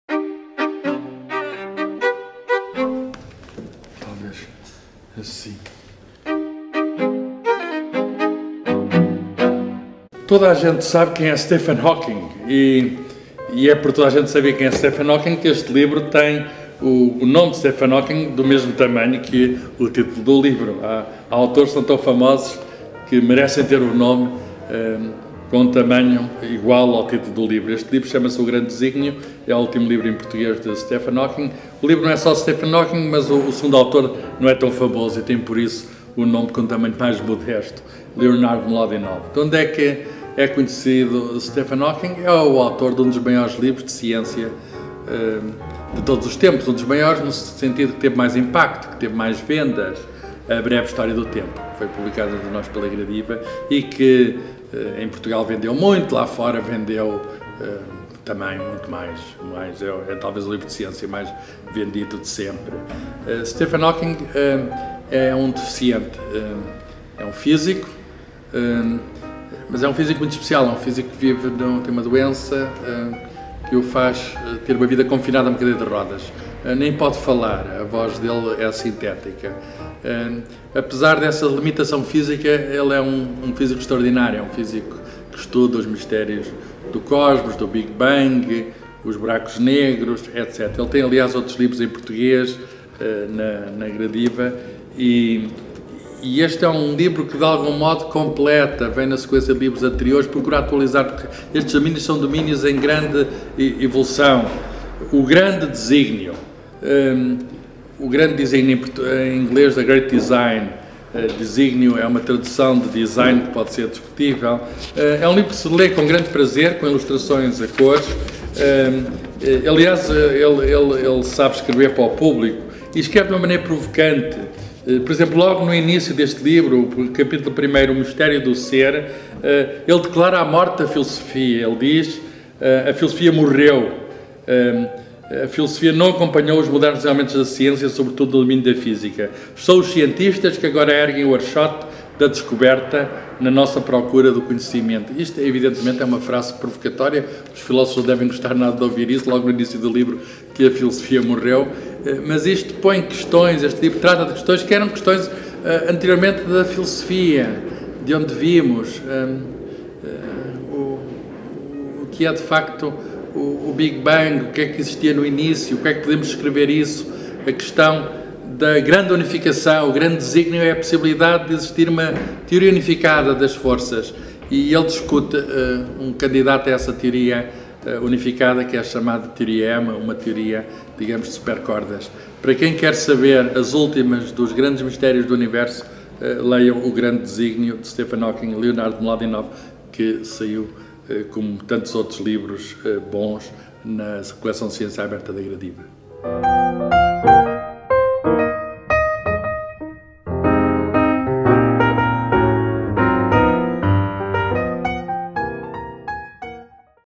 O Grande Designio
No âmbito das sessões "Café, Livros e Ciência" o Professor Carlos Fiolhais revela quais as suas escolhas de leitura. No mês de Agosto um livro do cientista britânico Stephen Hawking.